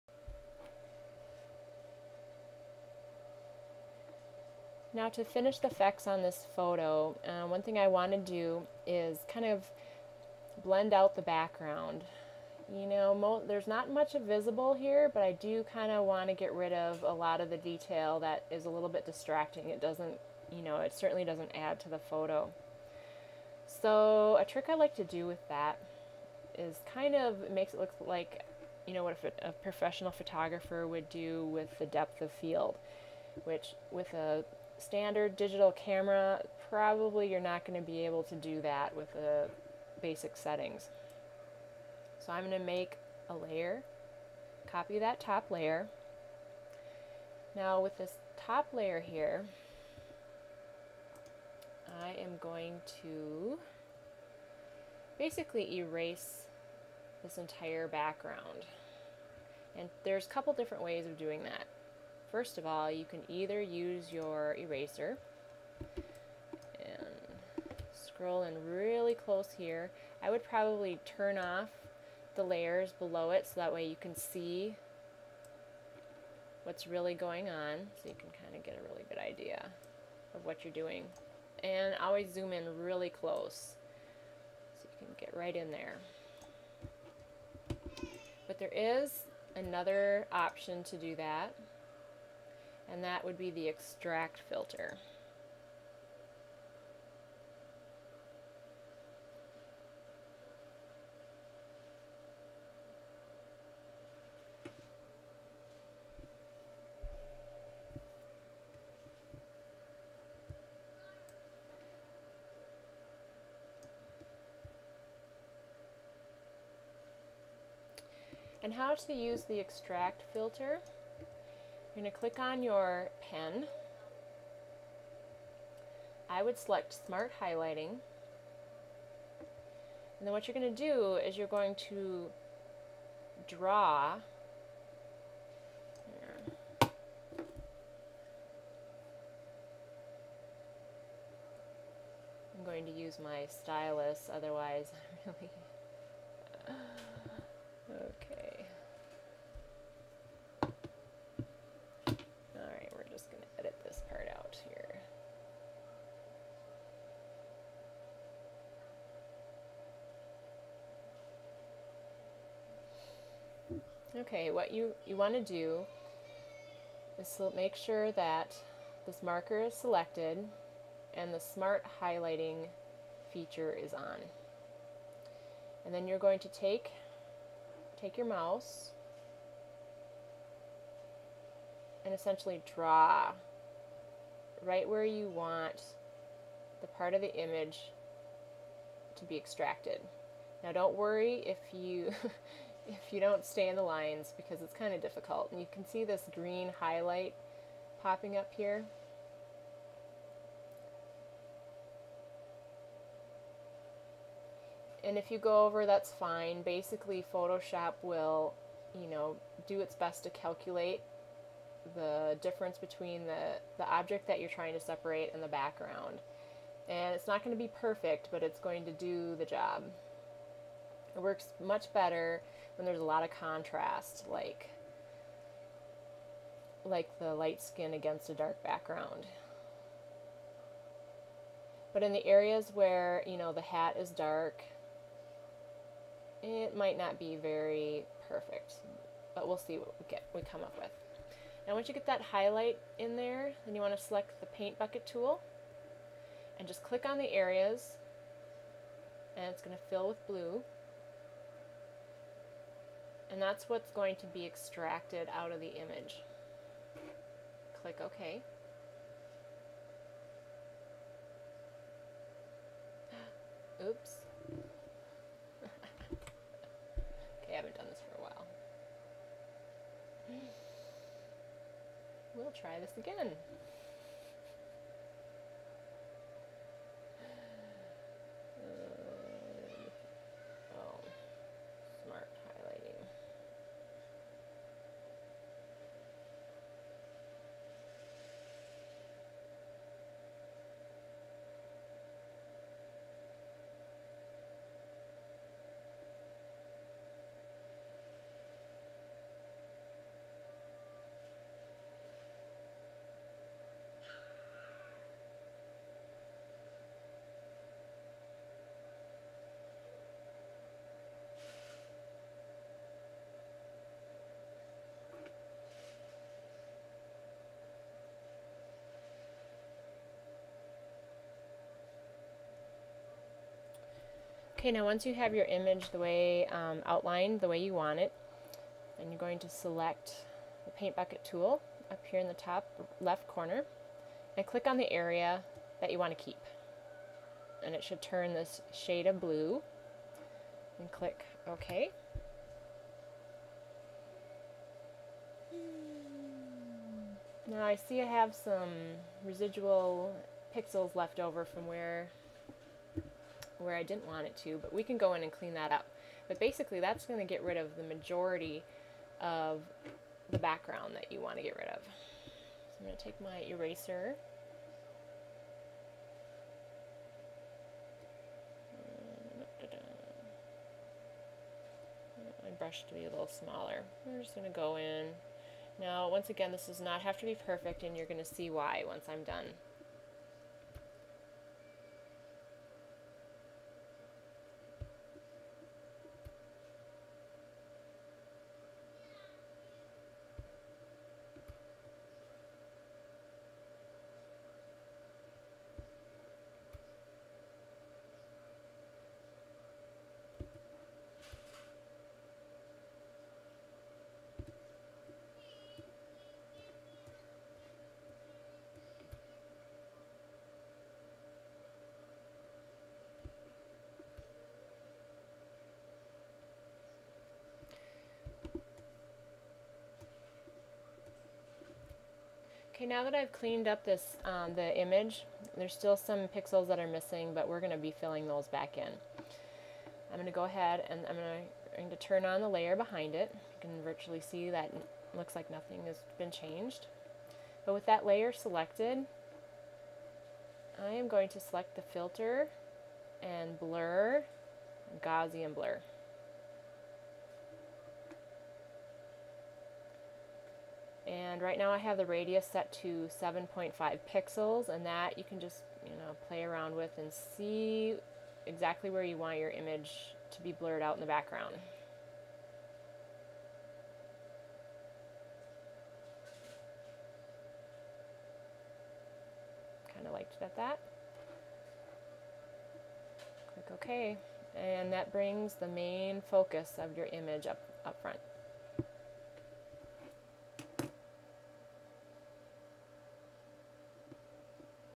Depth of Field
Here's a simple way to improve the composition of a photograph by adjusting the depth of field in Photoshop.